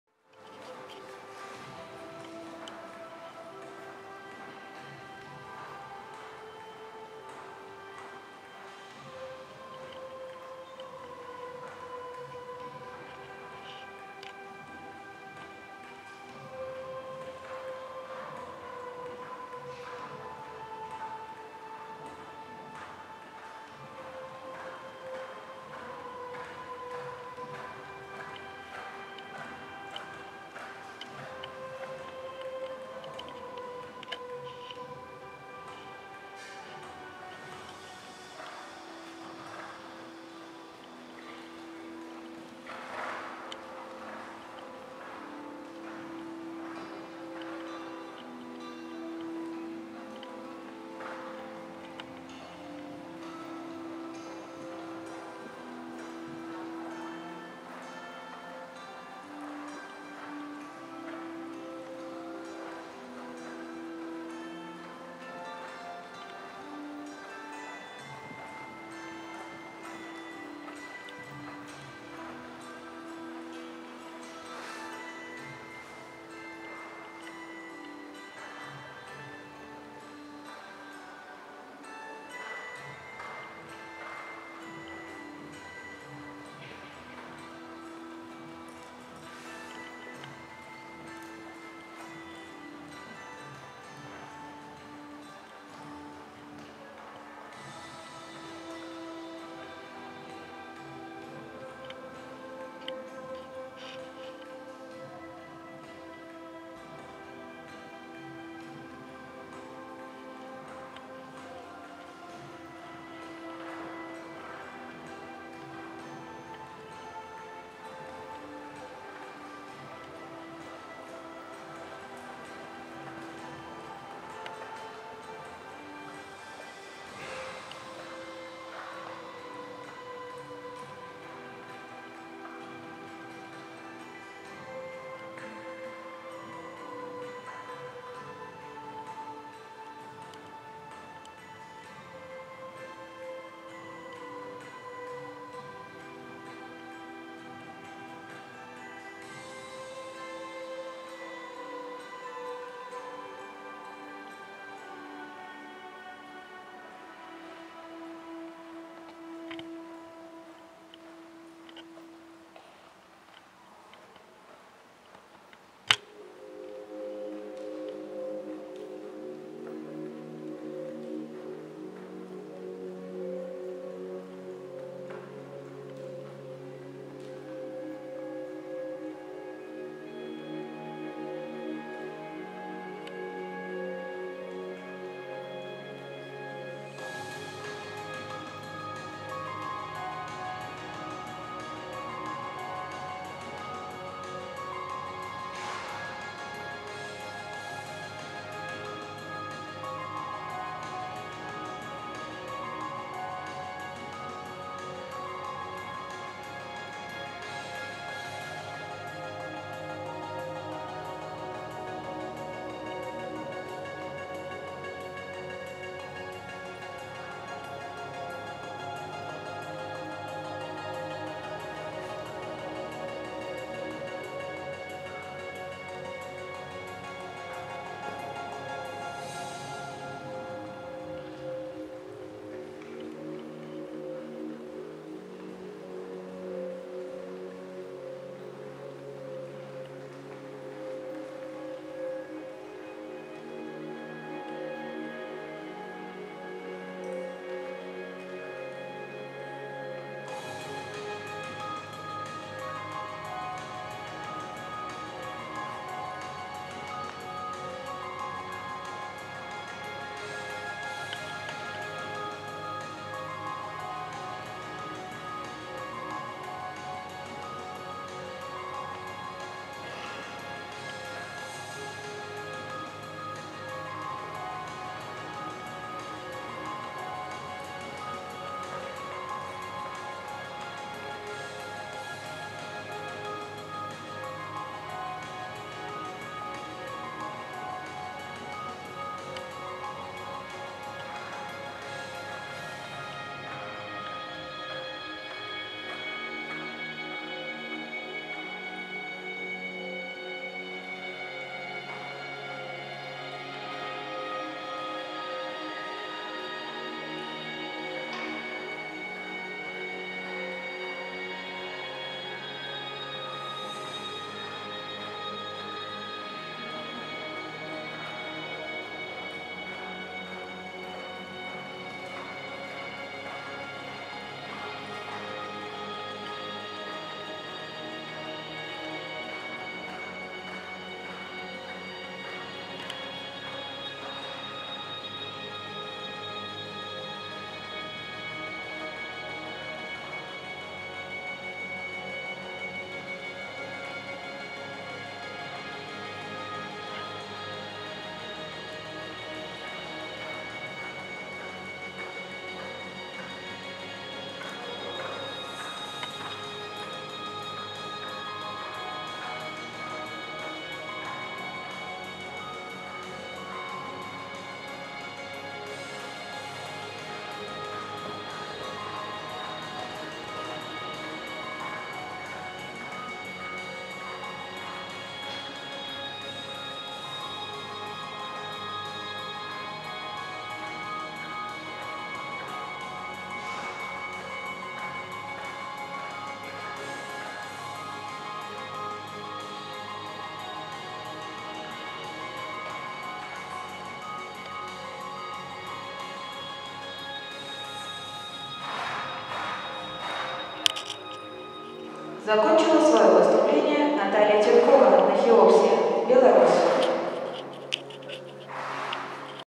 CDIJ. 62.424% Тюлькова Наталья и Хеопса (Беларусь)
Международный турнир по выездке, Минск 26-29 апреля CDIJ/CDIY/CDI2*/CDI3*
Результаты на сайте bfks.by и diamond-horse.com